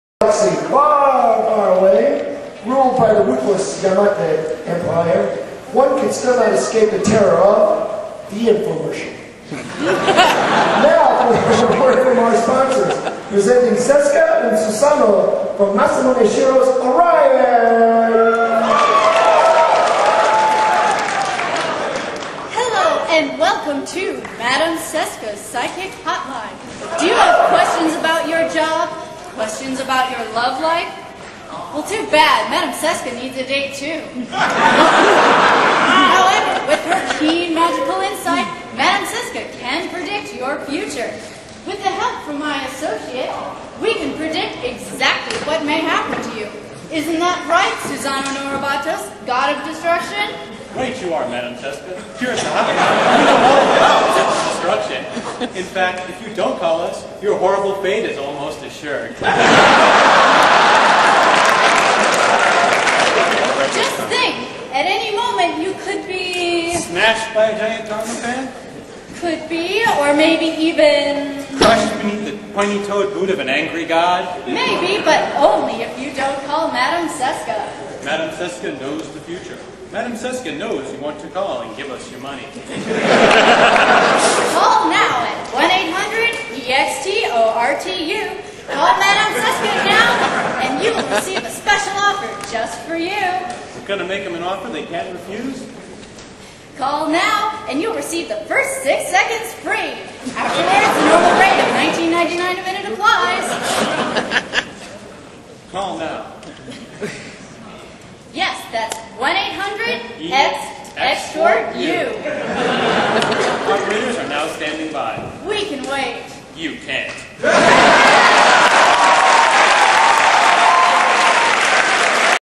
Erika Door @ Anime Central 2002 - #45 Seskas Psychic Hotline
Video of Entry 45, Erika Door cosplay skit of Madame Seskas Psychic Hotline from Orion performed at Anime Central 2002. Filming credits to Linus Lam at http:...